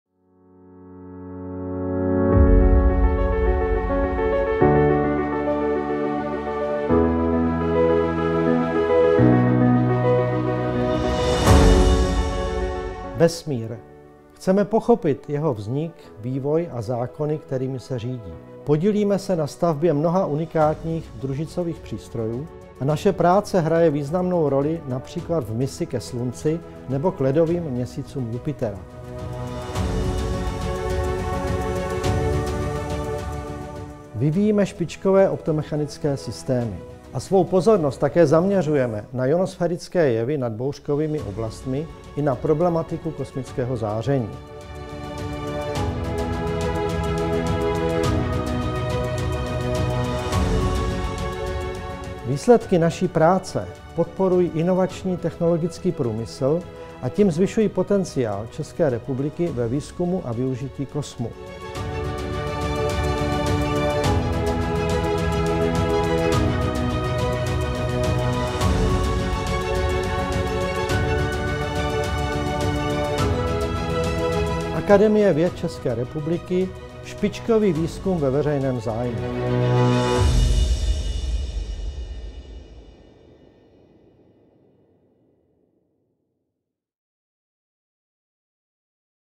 Strategie AV21 – Vesmír pro lidstvo
Program je zaměřen na posílení spolupráce mezi vědeckou komunitou a technickými týmy při vývoji a testování nových technologií kosmického výzkumu, zejména družicových přístrojů pro astronomická pozorování, která jsou klíčovým prvkem na cestě k hlubšímu porozumění fyzikální podstatě hmoty. Důraz je kladen i na transfer získaných technologií do aplikační sféry a na podporu inovačního technologického průmyslu, který je na kosmický výzkum navázán. Témata programu zahrnují např. účast na velké rentgenové misi Athena, na misi k ledovým měsícům Jupitera (JUICE), na přípravě návrhu polarimetrické družice XIPE, na projektu evropské vesmírné mise ke Slunci (Solar Orbiter), na vývoji špičkových optomechanických systémů pro družice či družicový výzkum nadoblačných výbojů.